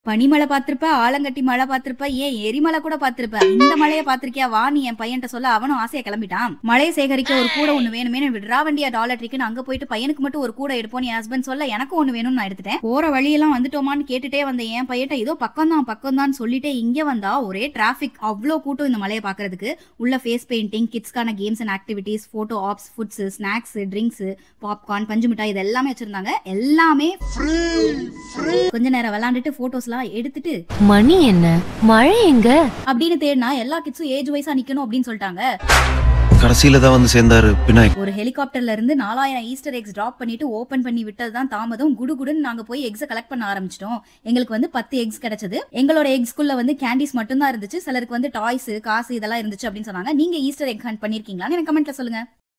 💥Americaல இப்படி கூடவா🥚மழை வரும்⁉️💢
This tamil shorts video is about me going to an event which started in the city of San Antonio last year. We went to the part where the event was happening. The parking lot was full when we entered and there was no place to park the car. Most of the cars where parked in the roads and the grass. We had to bring baskets to the event. They had free food, drinks,snacks, popcorn, cotton candy, face painting, photo ops and lots of kids activities. Finally the moment arrived and we were all so happy to see the egg dropped from the sky through helicopter.
I share tamil shorts videos and usa tamil vlogs regularly .